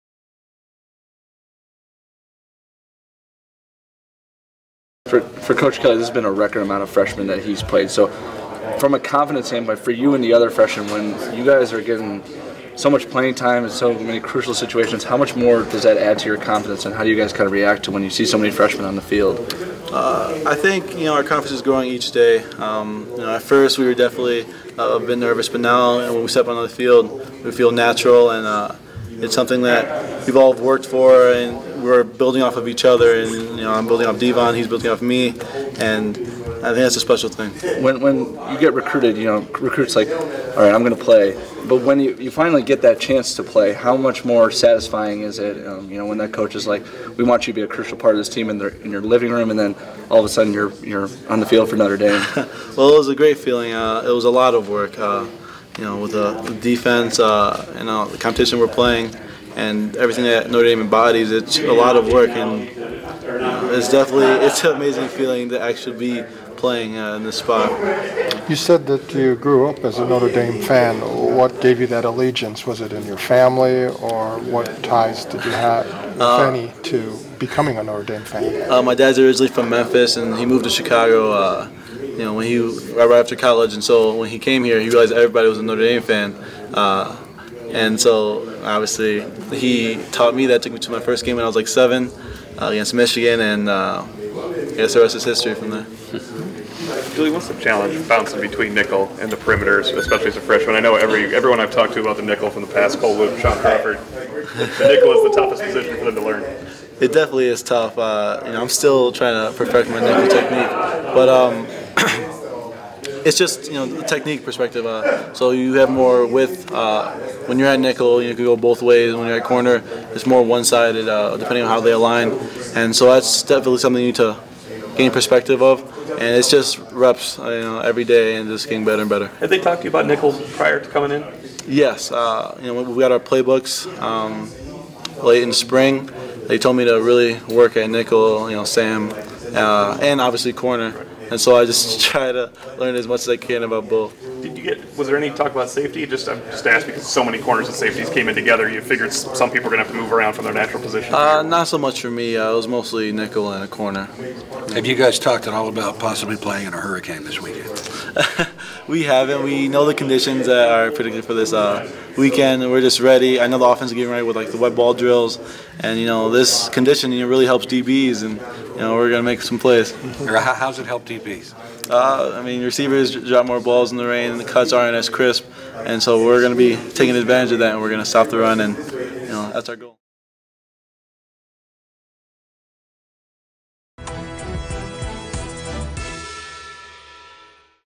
Julian Love perfecting his technique
Julian Love talks about moving into the rotation in Notre Dame's secondary as a true freshman.